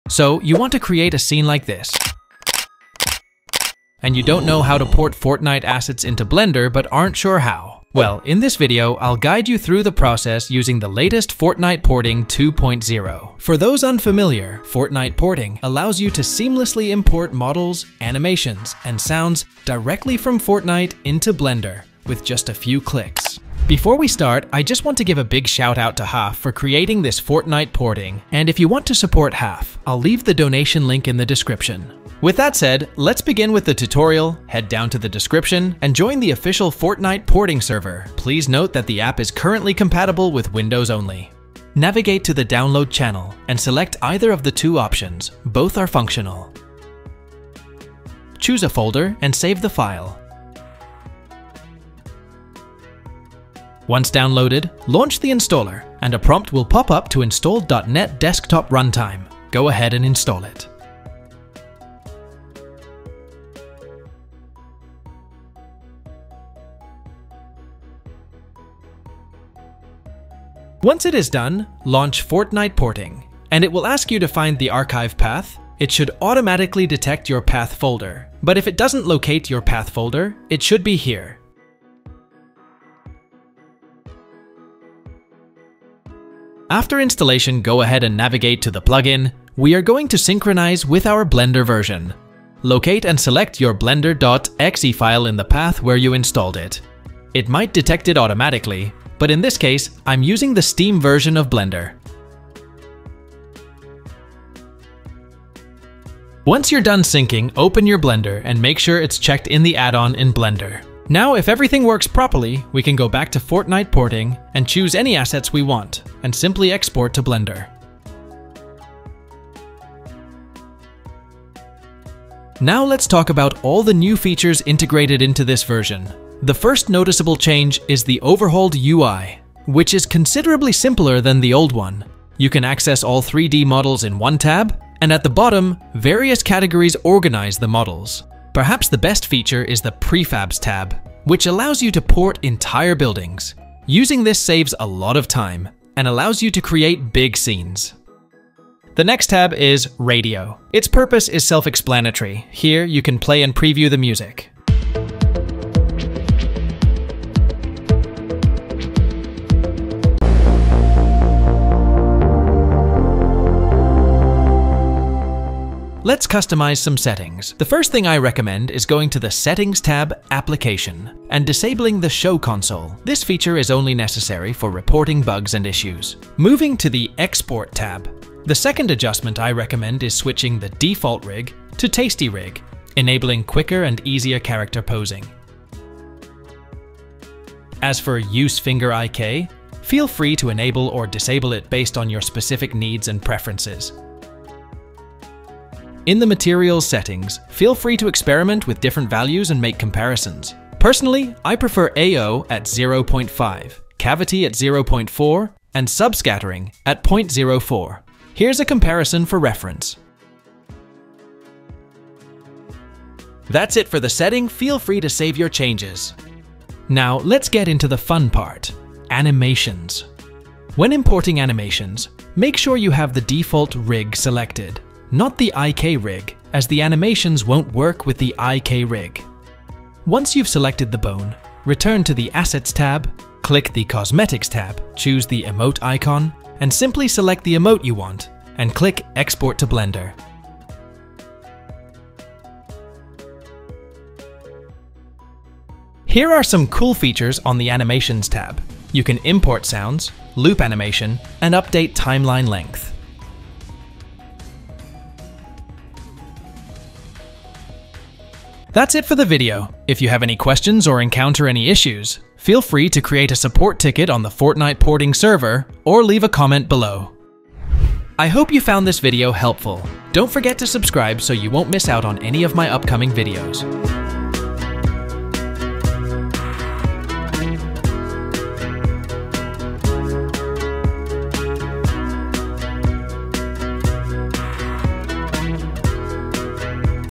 How to Port Fortnite Models in ONE CLICK - 2024
In this video, I will show you how to port Fortnite models To Blender Using Fortnite Porting 2.0 in one click.

NOTE: Ensure you have Fortnite Installed

Chapters
00:00 Intro
00:32 Installation
02:02 New Features
02:46 Settings
03:37 Animations
04:18 Support & Outro